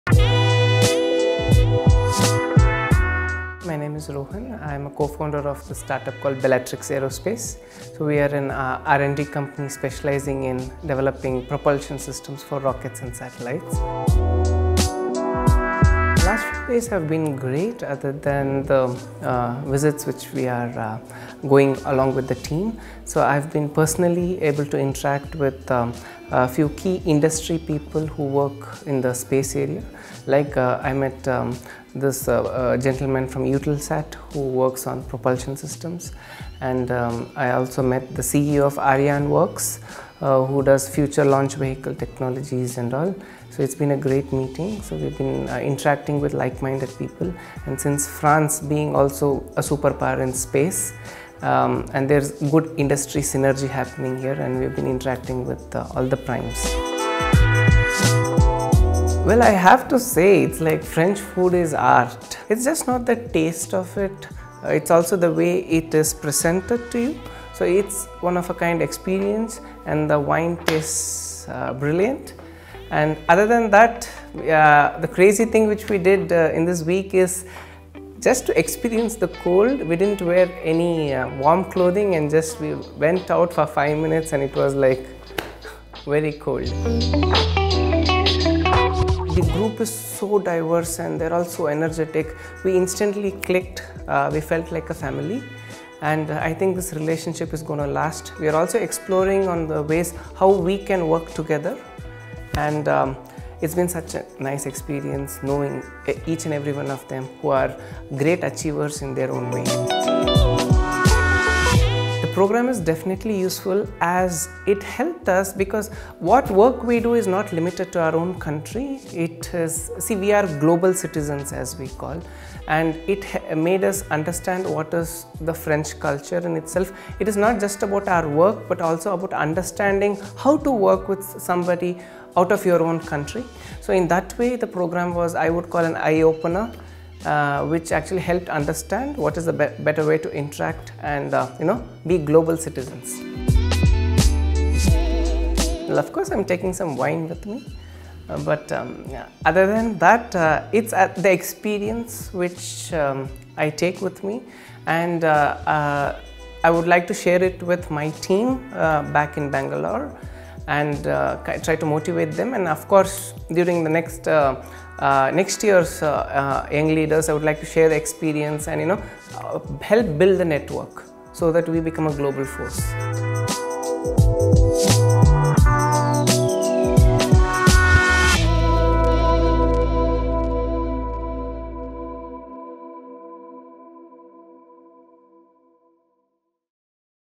Young Leader - Rohan Ganapathy
Club Young Leaders India-France 2019 brings together 10 promising personalities, who have proven themselves in various fields such as aeronautics, investigative journalism, baking, renewable energy, sustainable architecture, etc.

Founded by the President of the French Republic, H.E. Mr Emmanuel Macron, during his State visit to India in 2018, this Club aims to enhance France’s attractiveness among its primary ambassadors.
Rohan Ganapathy, 26, (CEO) Bellatrix Aerospace, is the next-gen rocket man.

Since decades, no revolutionary technology has been found to improve engine efficiency in rocket propulsion systems. Rohan has the mettle to experiment on paths that have never been taken in the West. He is at the cutting edge of innovative research on rocket engines and satellites. He has invented a plasma propulsion engine and green fuel; his company, Bellatrix Aerospace, is capable of reducing costs for a mission thanks to its patented propulsion systems in orbit. CNES is developing a partnership with him to inject his innovative skills in future launchers, the forerunner of the post-Ariane 6.

Bellatrix Aerospace plans to establish facilities in Paris, France.